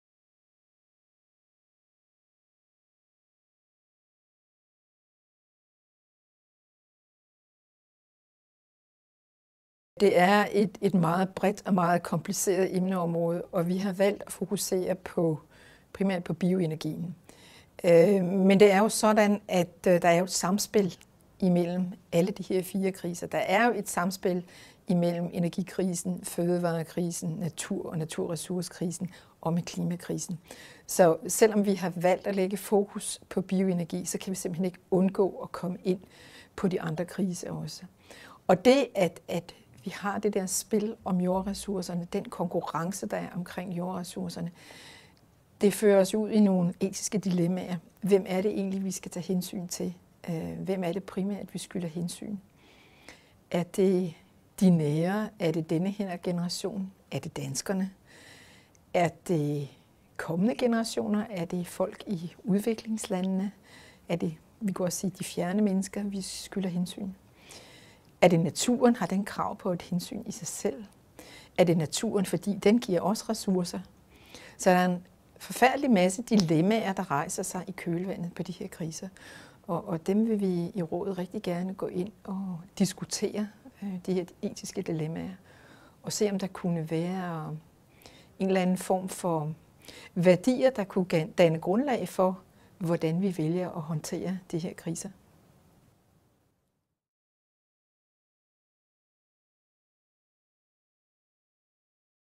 Hvordan tilgår Det Etiske Råd projektet om bioenergi, fødevarer og etik?
Rikke Bagger Jørgensen er formand for Det Etiske Råds arbejdsgruppe om bioenergi, fødevarer og etik. I videoklippet fortæller hun, hvordan Rådet tilgår projektarbejdet og hvillke etiske dilemmaer emneområderne berører.

Rådet afholder en debatdag -- Bioenergi, fødevarer og etik i en globaliseret verden -- på Christiansborg, den 10. maj 2012. Læs mere på